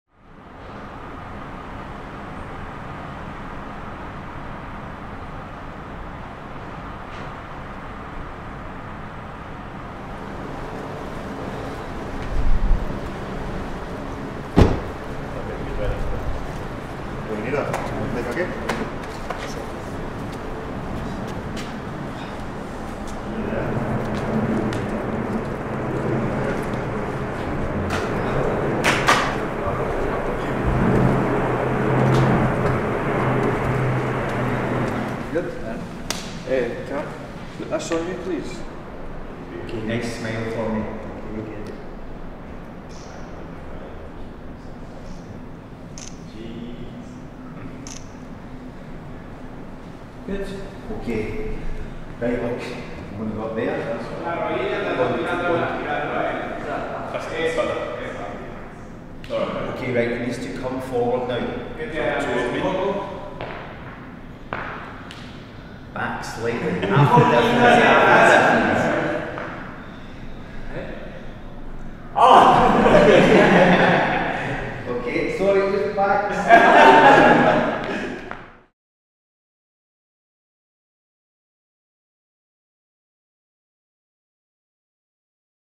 TRAILER | Óscar Cortés Signing | Behind the scenes with our latest arrival | 01 Feb 2024
Go behind the scenes as Óscar Cortés signs for Rangers from RC Lens.